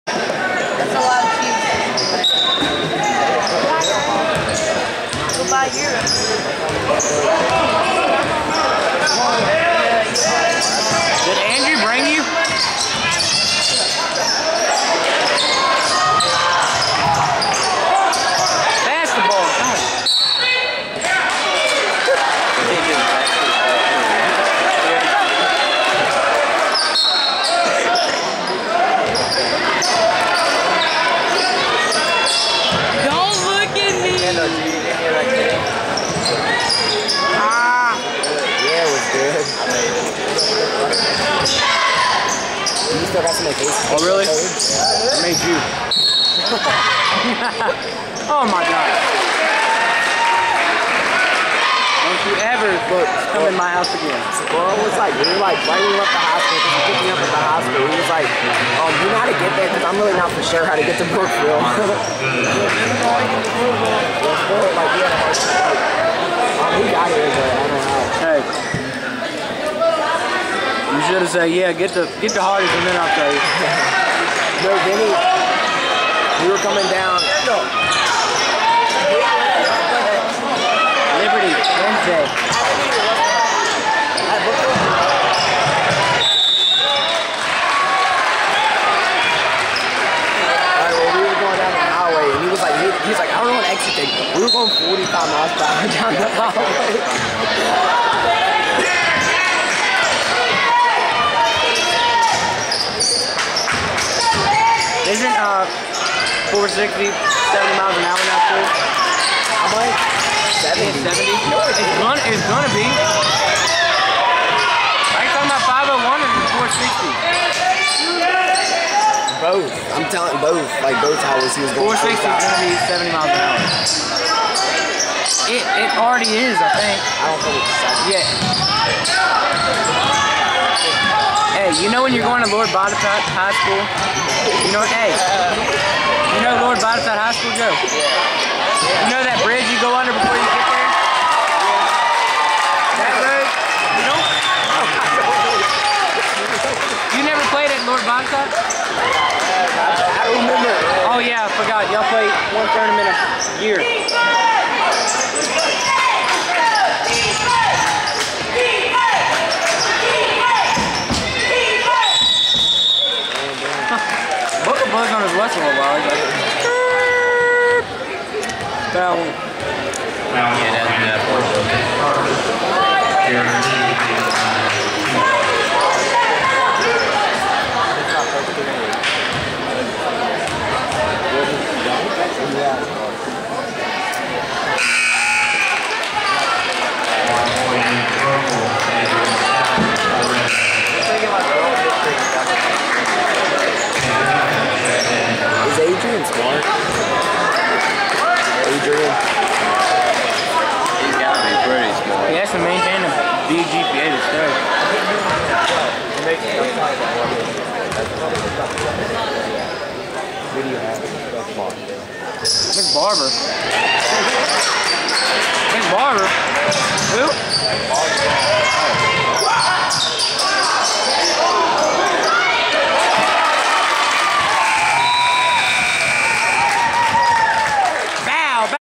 Rustburg vs. Brookville 2/14/11
Rustburg 58 Brookville 34
Marshall Wood 18pts, 10rebs, 4assts, 3 steals